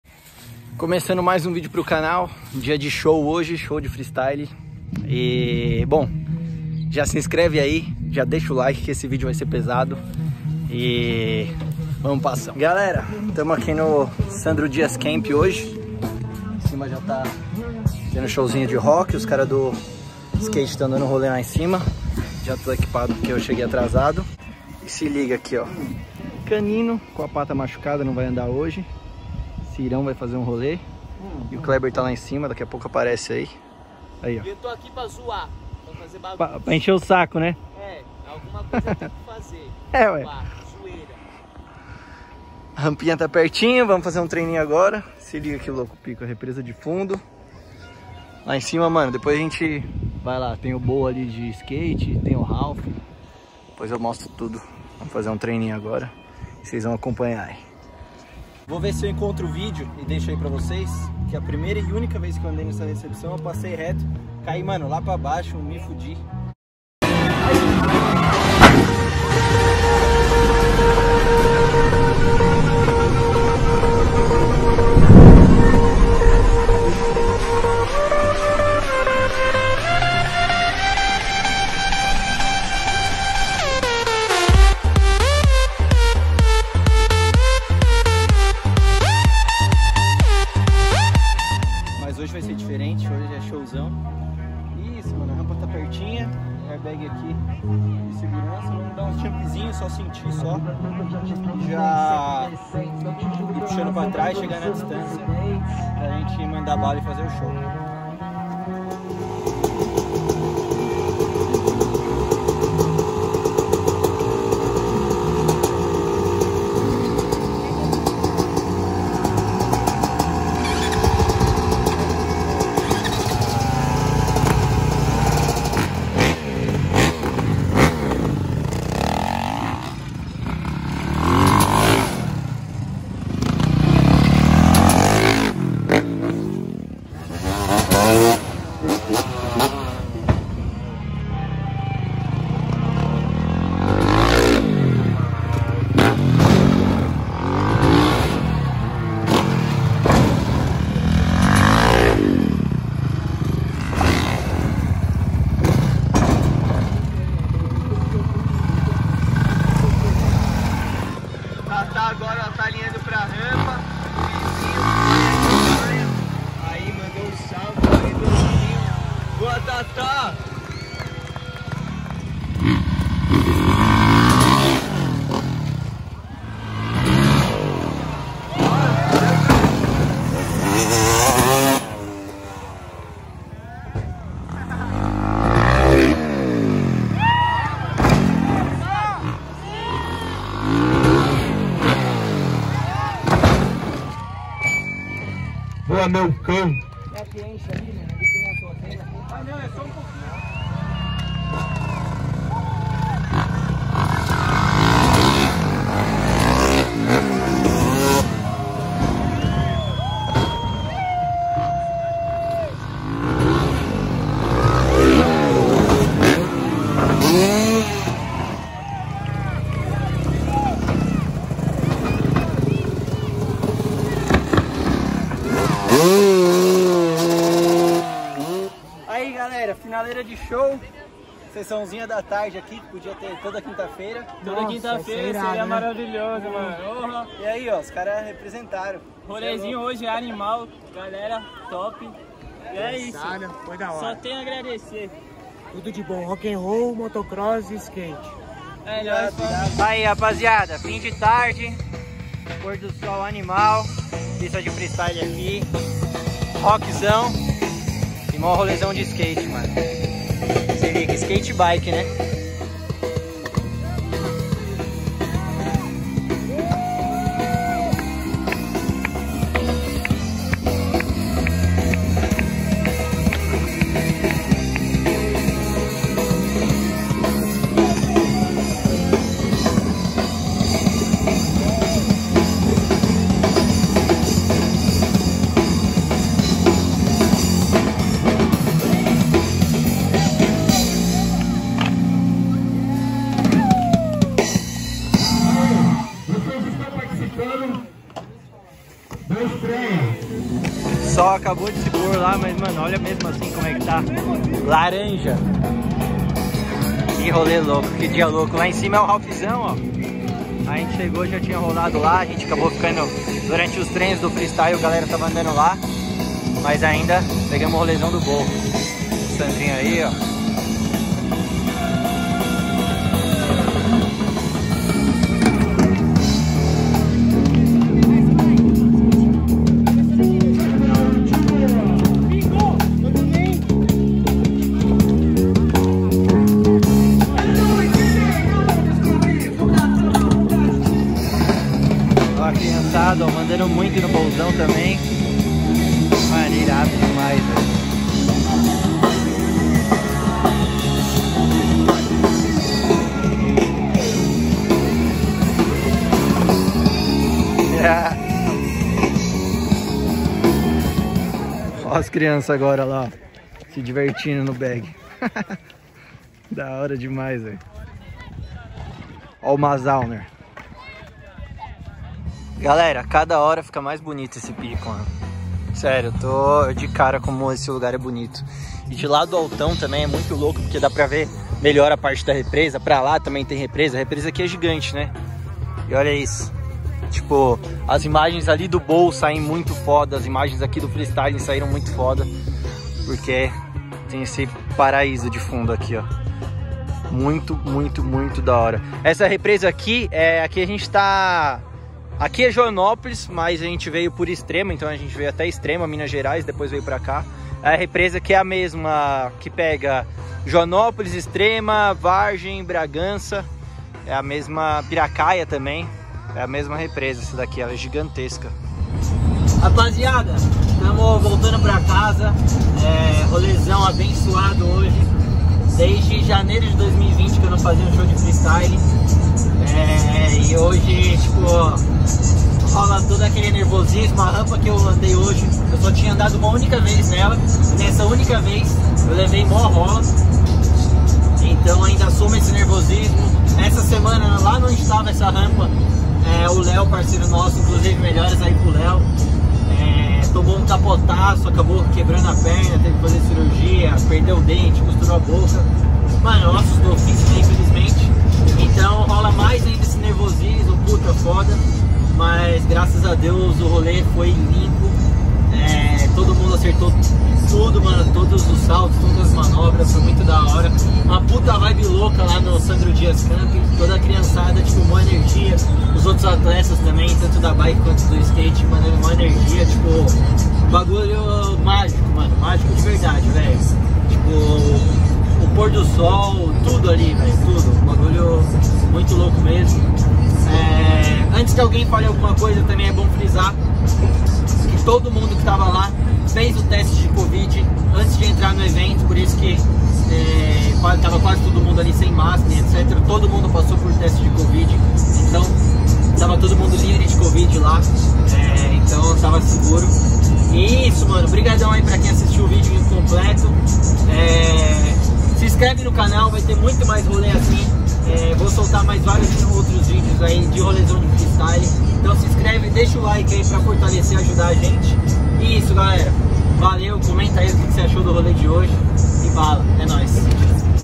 Show de FreeStyle Motocross no Sandro Dias Camp
Show Irado de Freestyle Motocross que fizemos no Sandro Dias Camp!!

Vibe animal, e galera mandando muito na moto, bike e skate.